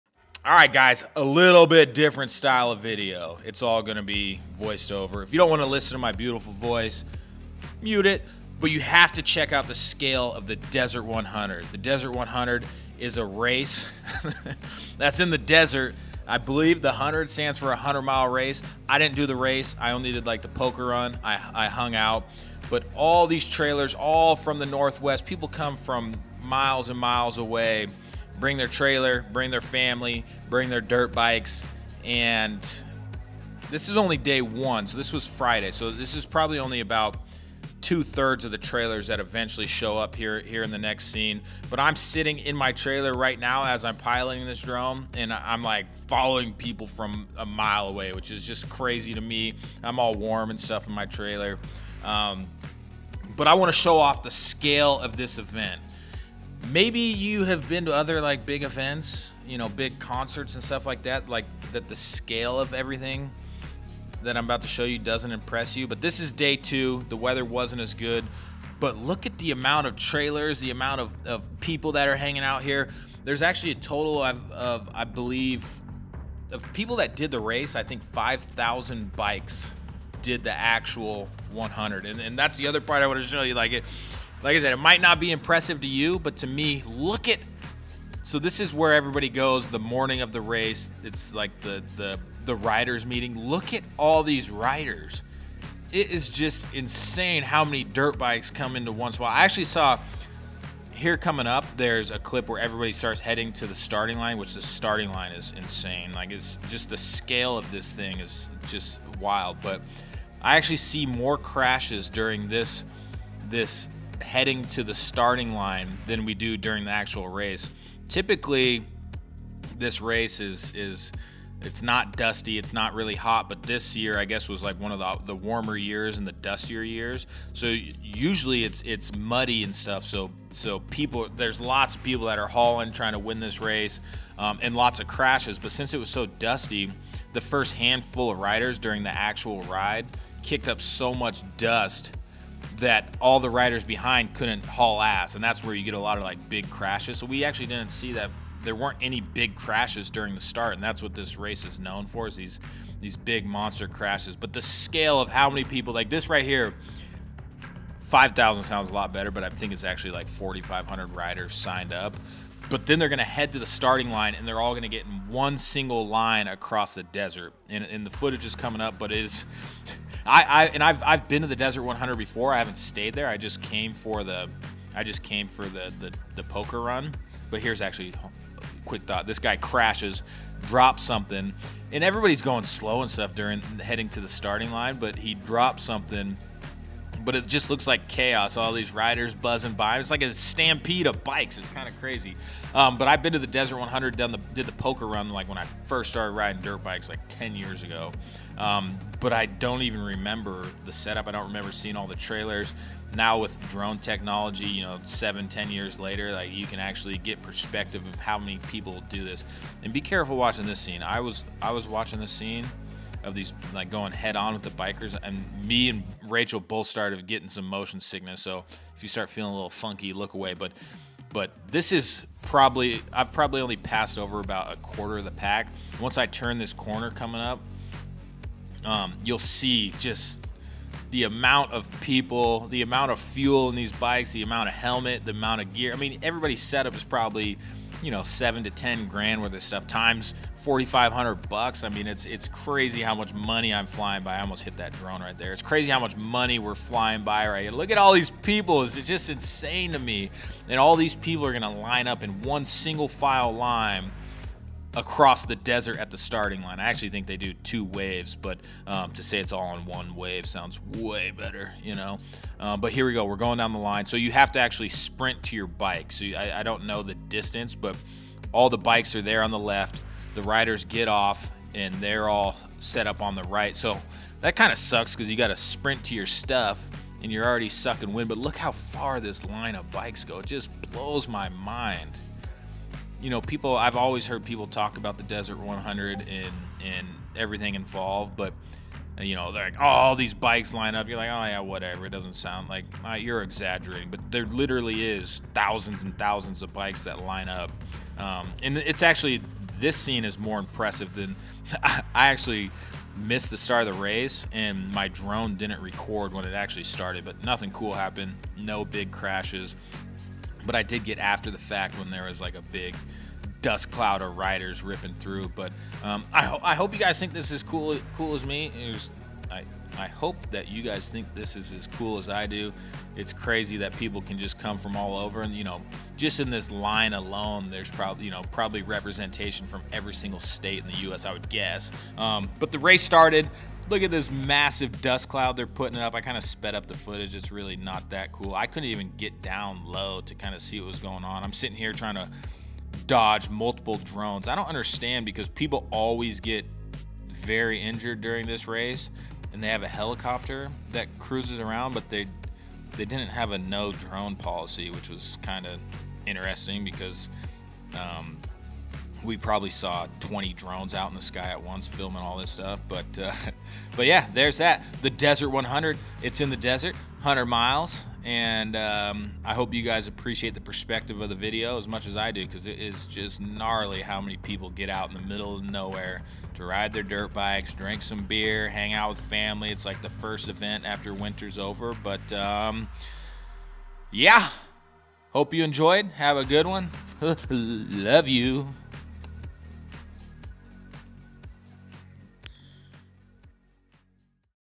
What Does 5,000 DIRT BIKES Lined in a Row Look Like? 2022 Desert 100 Race
I decided to head to Odessa WA where they host the Desert 100 race. This was their 50th anniversary of the event. I have always heard it was an insane race but I needed to see the start of the race for myself. We packed out our trailers and met at the event with some other friends. The race was cool but you get to only catch the start. What really blew my mind was the scale of the event and how many trailers and bikes are squeezed into a desert in the middle of no where. Have you ever wondered what 5,000 dirt bikes lined up in a row looks like? Well here you go!